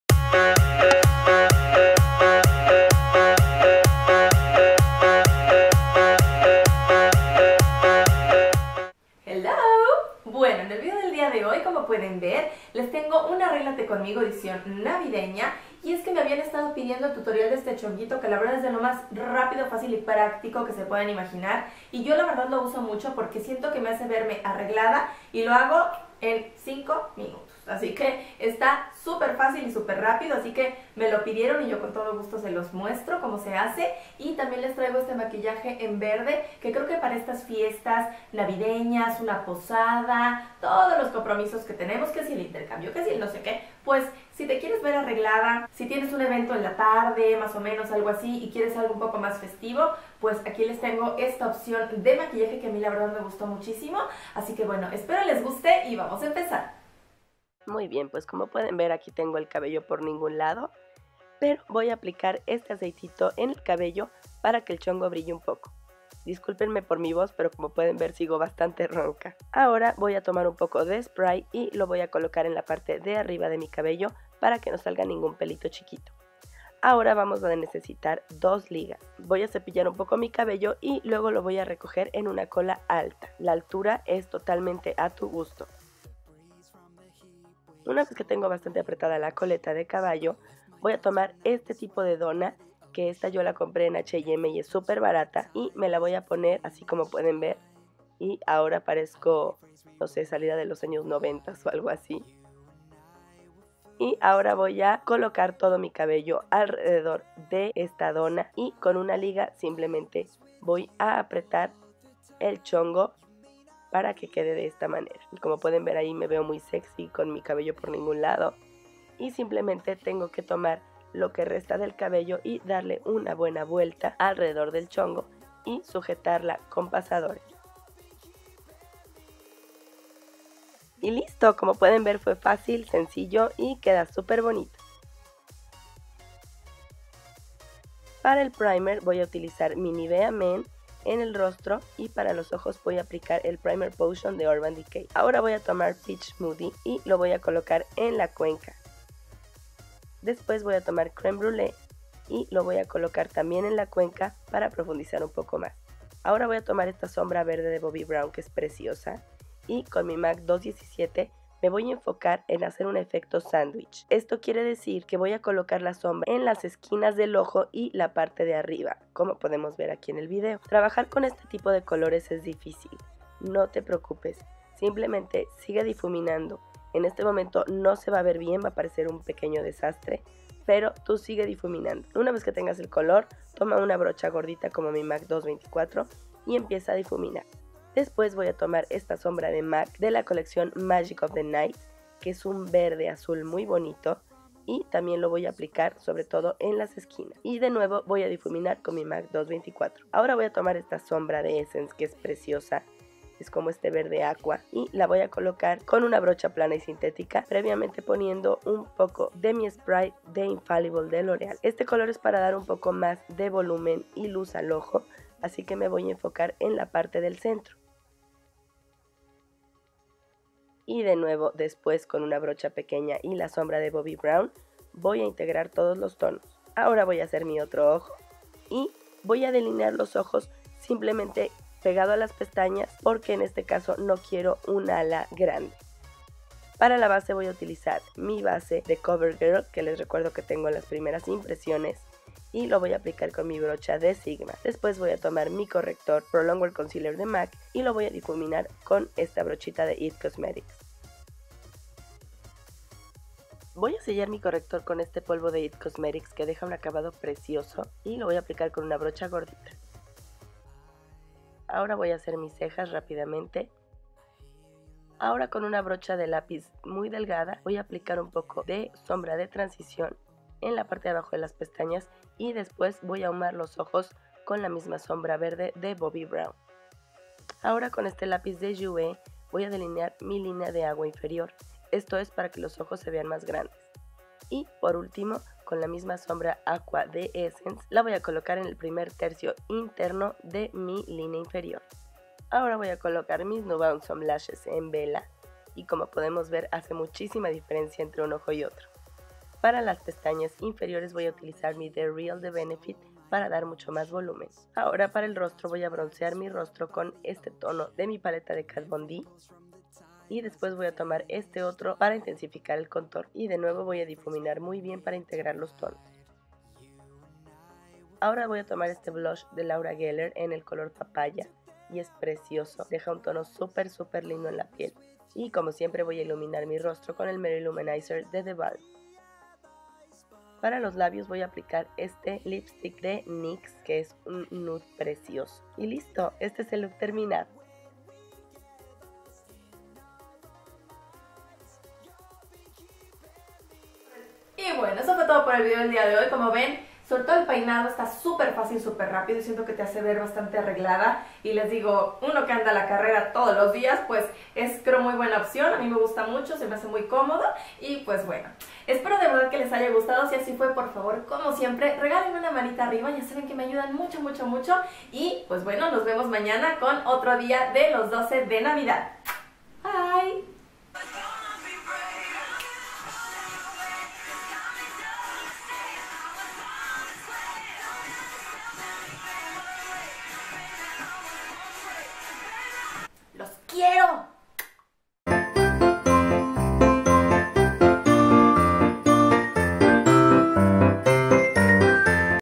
ARREGLATE CONMIGO EDICION NAVIDEÑA Maquillaje verde para Navidad!
❄ Hola mis amores! En este video les muestro cómo hacer un maquillaje verde para navidad y cómo hacer un chongo casual ¡Espero les guste! Este es el cuarto video de 12 que les tendré, hasta el 24 de diciembre!❄

Sígueme!